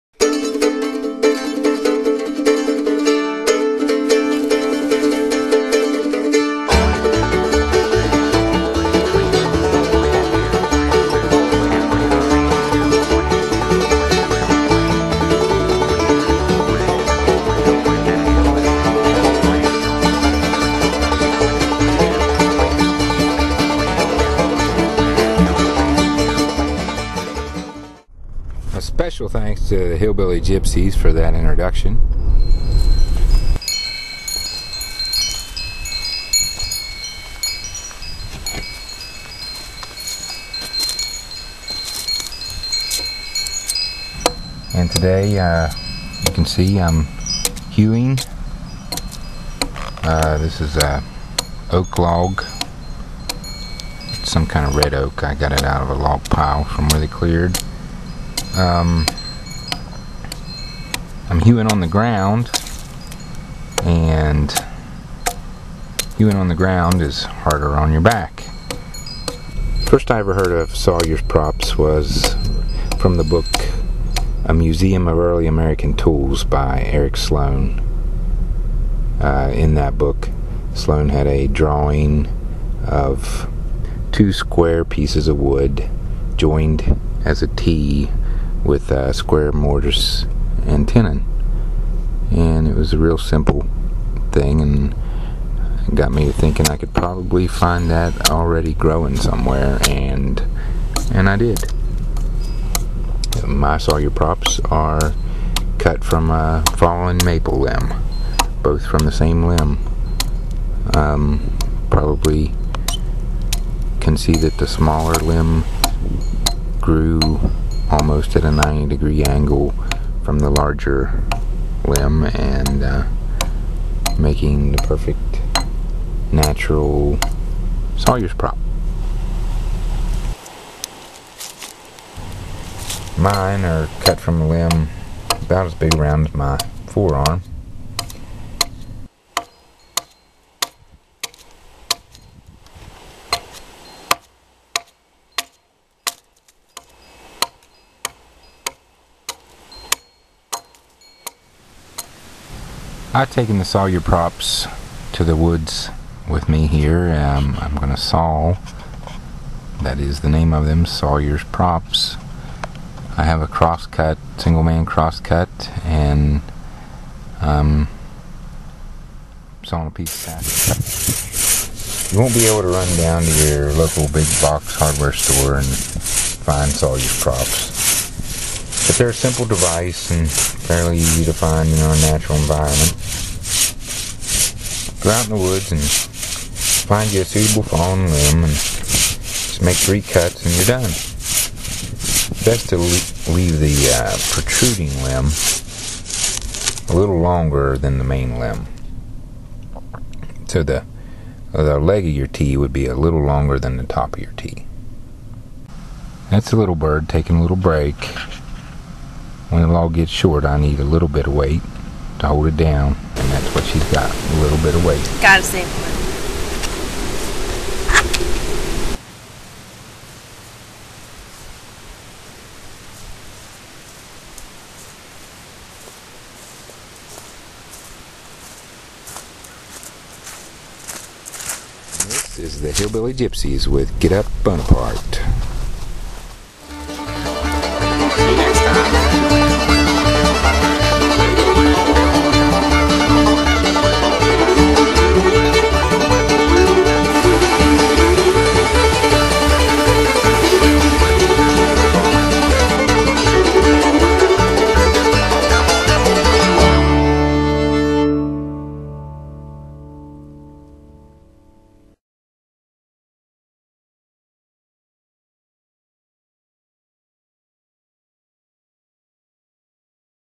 Sawyers Props.wmv
Sawyer's props, are simple devices that can be fashioned from fallen limbs and assist you in getting your work up off the ground.